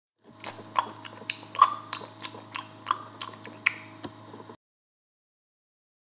1 AN de JEAN CHIBRE ! MERCI A VOUS !
Laissez moi vous expliquer : cela fait 1 an que je change la face du monde grâce à mes tutoriels incroyables, mes parodies d'anthologie, mes rushs inédits montrant des personnalités nues, mes fameuses YTP et, plus dernièrement, mes POLPORNs :).
Merci à vous de me soutenir, l'aventure continue !